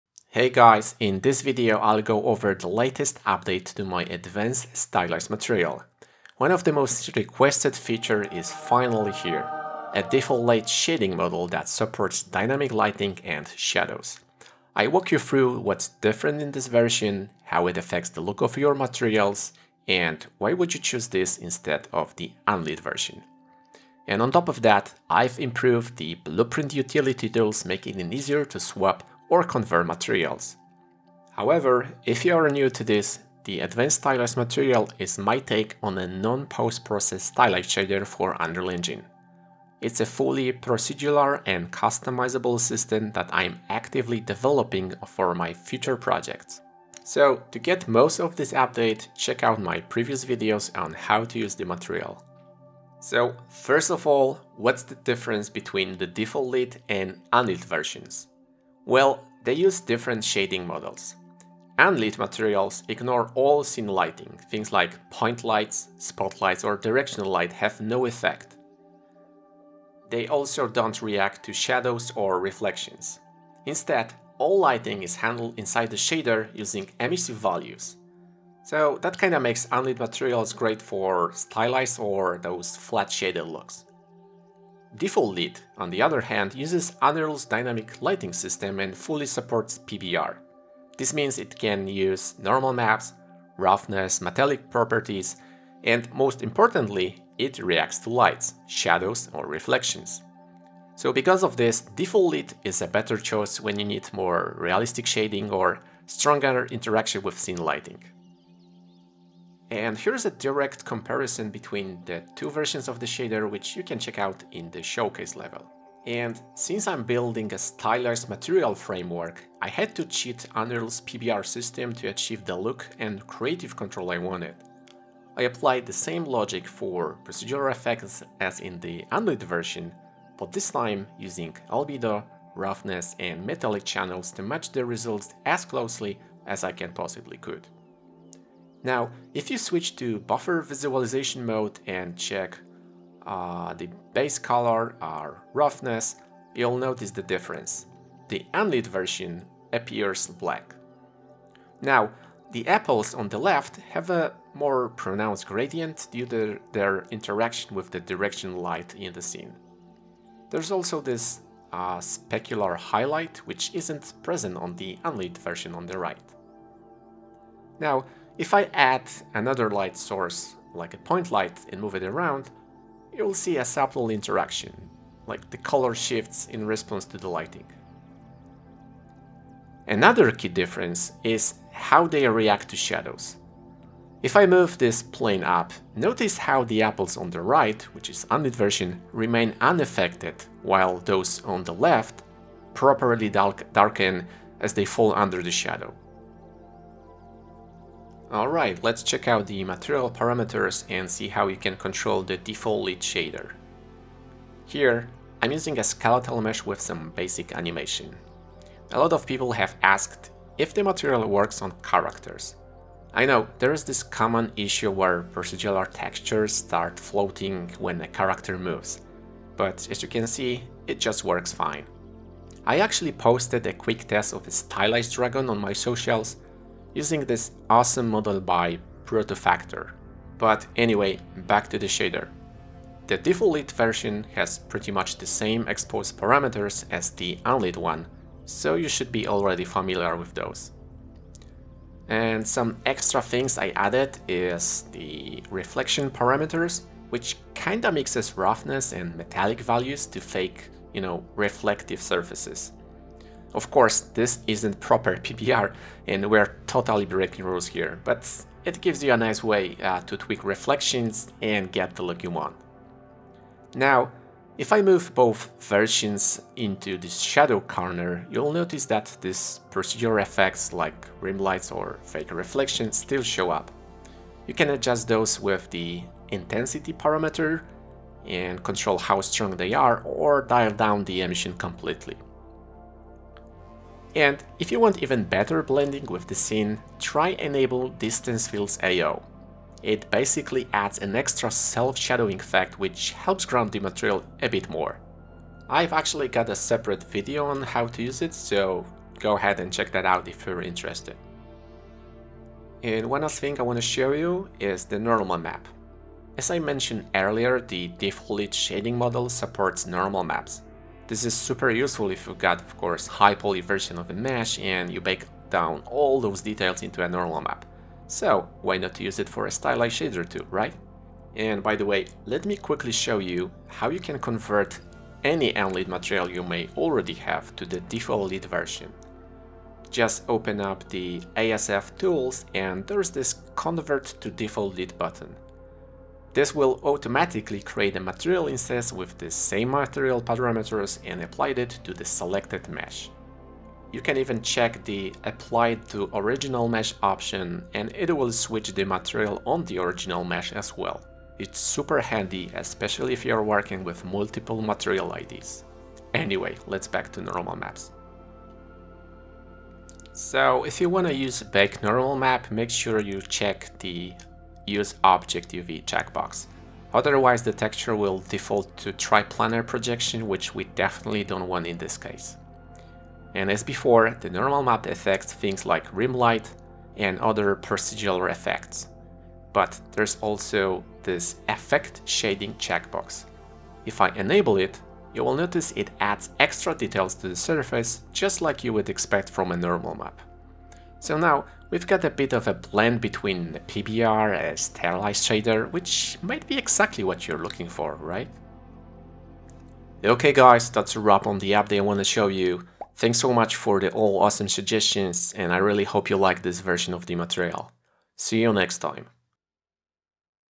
Stylized Material UE5: Default Lit vs. Unlit
In this video, I walk you through the latest update to my Advanced Stylized Material Pack, introducing the new Default Lit version, explaining how it differs from the Unlit shader, showcasing normal map support, lights interaction, and demonstrating how to convert existing materials with ASF Tools.

Time stamps
00:00 Intro
00:54 The difference
01:49 Direct comparison
03:16 Material Parameters
05:03 Normal Map
06:39 Outro